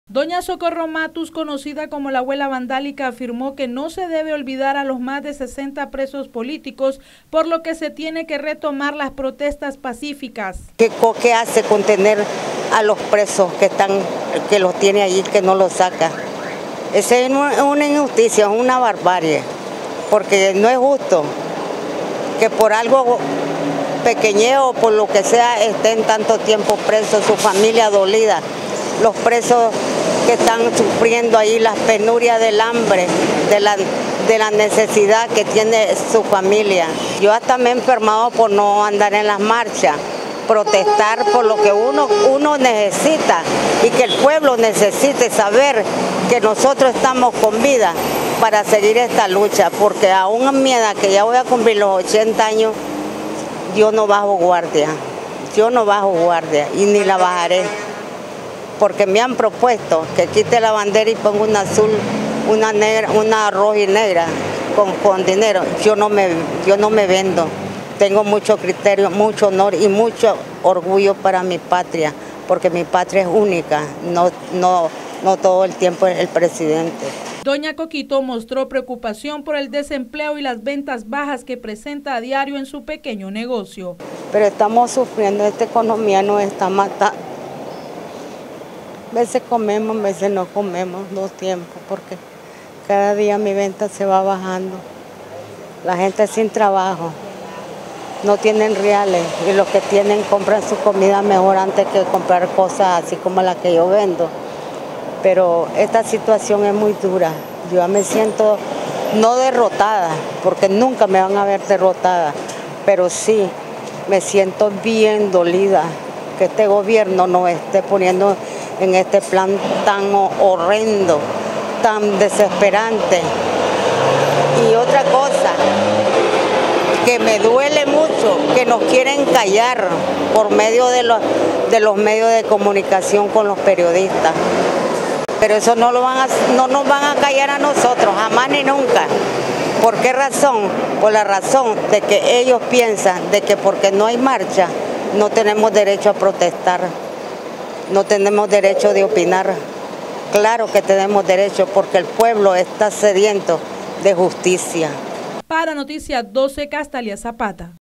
“No debemos olvidar a los más de 60 reos políticos”
Doña Coquito conocida como la abuela vandálica dijo que no debemos olvidar a los más de 60 reos políticos.